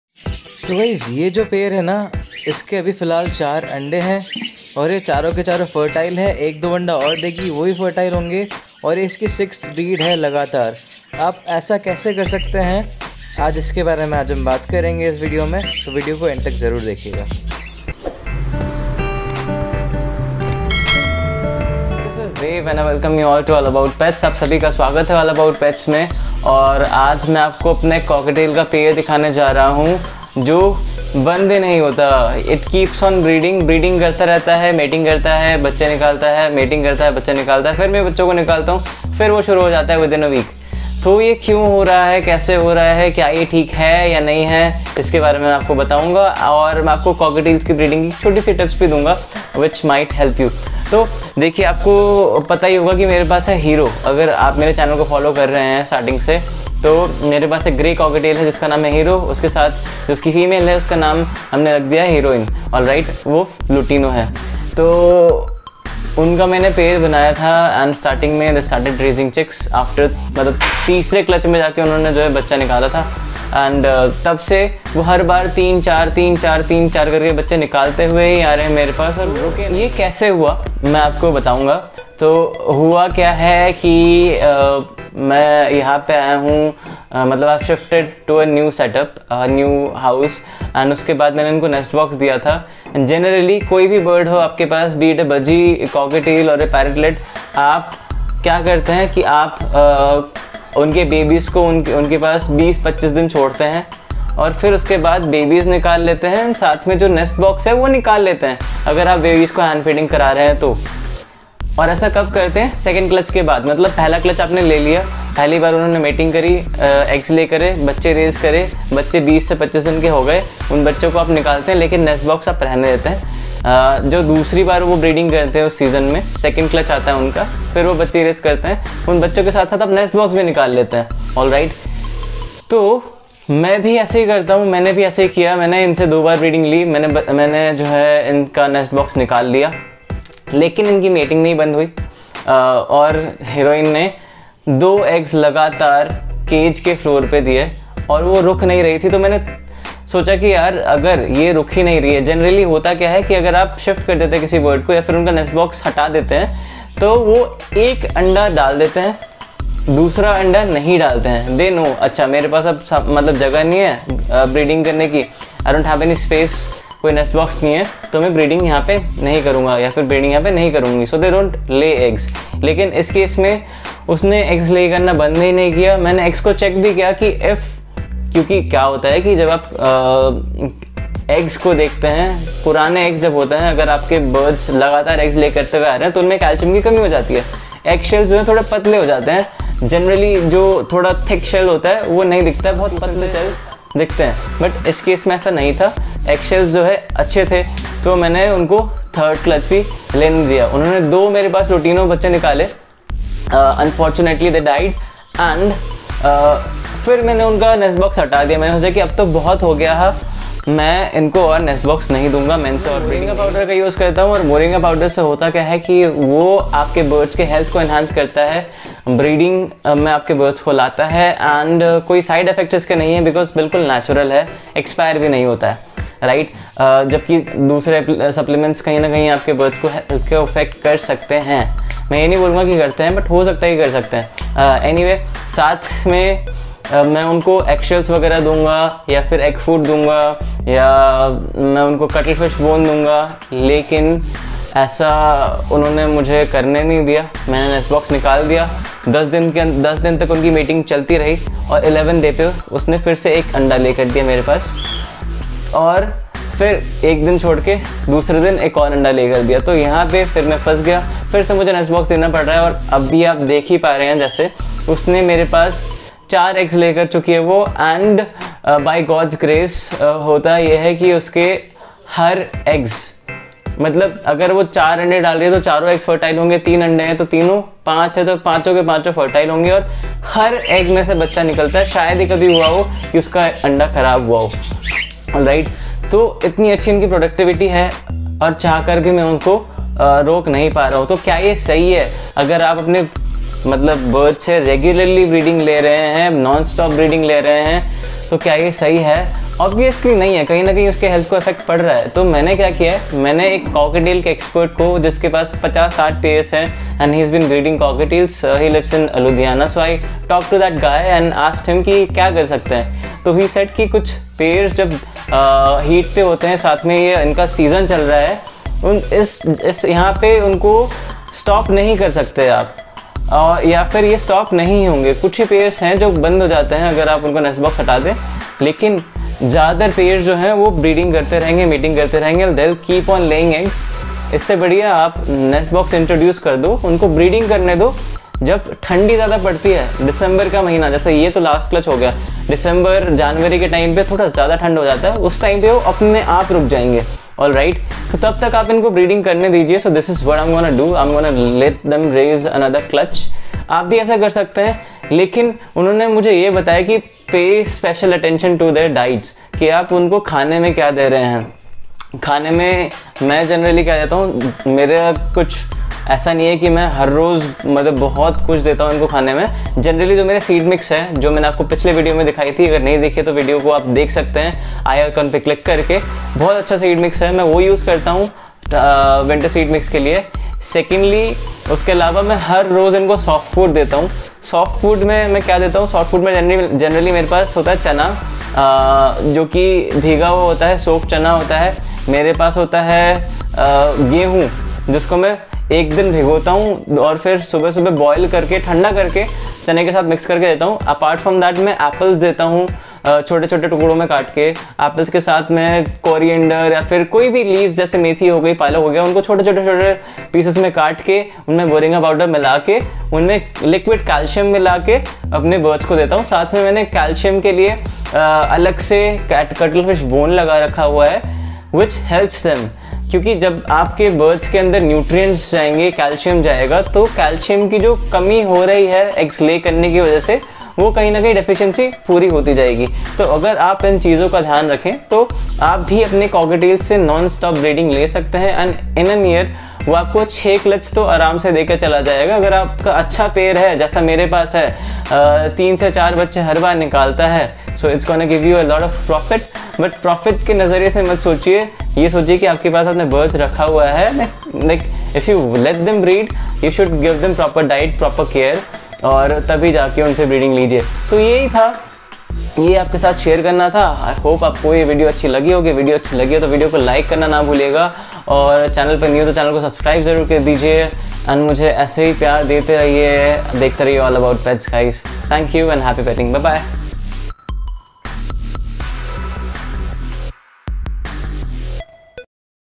Taking 6 BACK TO BACK CLUTCHES from Cockatiel Pair | यह जोड़ी कभी नहीं रुकती | All About Pets
Hey there, welcome to All About Pets. In this video, I've shared my insights on a pair of cockatiels who keeps on breeding and has produced 6 successful clutches. How did do this? To find out, you need to watch the complete video.

Contact All About Pets -